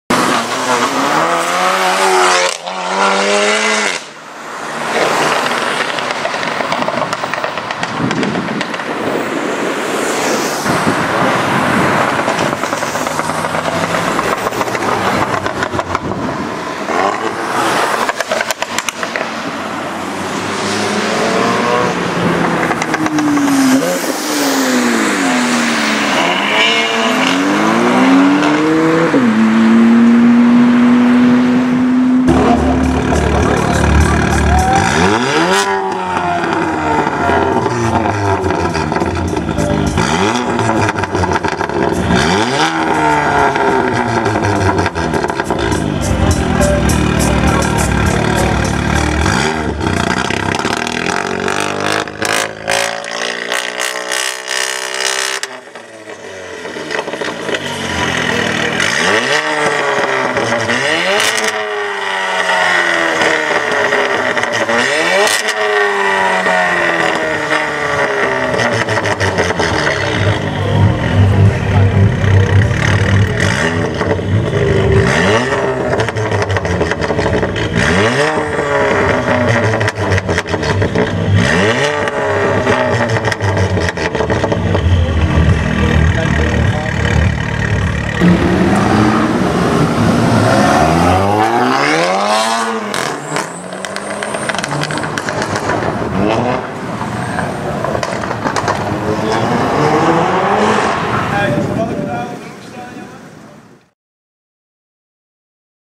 Seat Leon Cupra 300 w/ Straight pipe Milltek Exhaust - Loud Revs, Accelerations & Crackles!
In this video you see a Seat Leon Cupra 300 with a pretty loud milltek exhaust system.